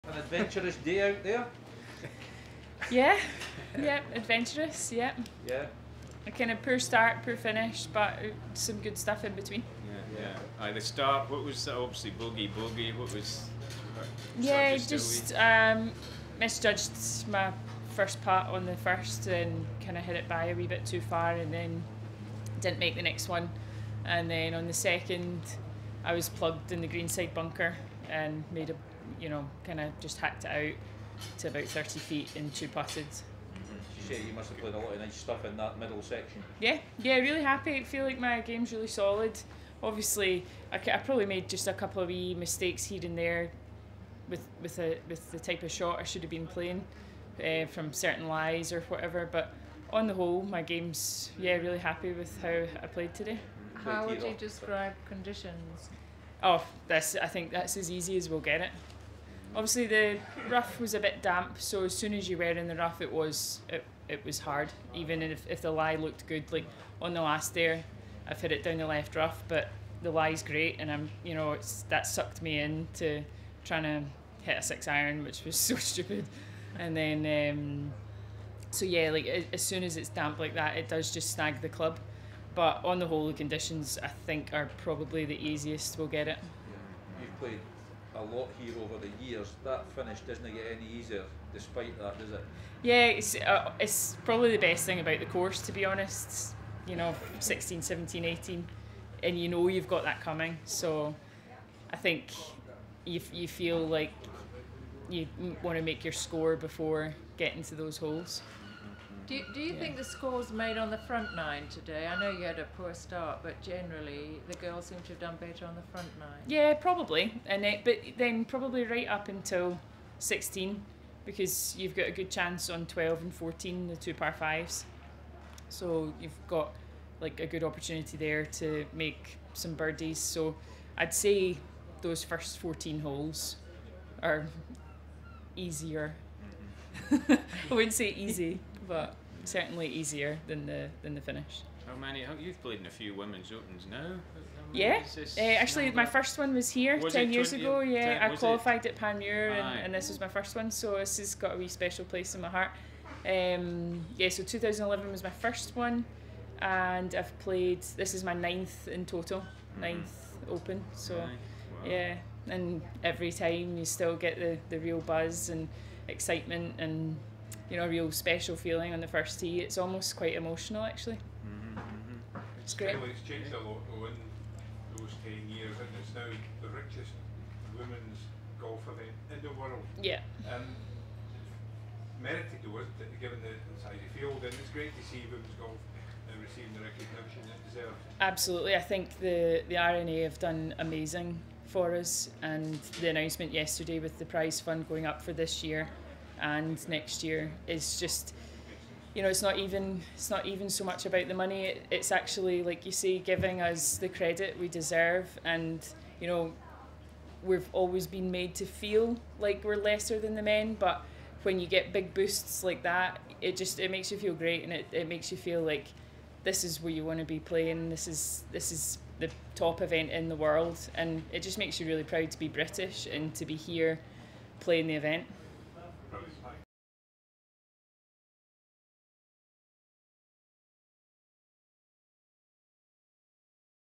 Kylie Henry Thursday Press Conference 2021 AIG Women's Open
Kylie Henry Thursday Press Conference 2021 AIG Women's Open © AIG Women's Open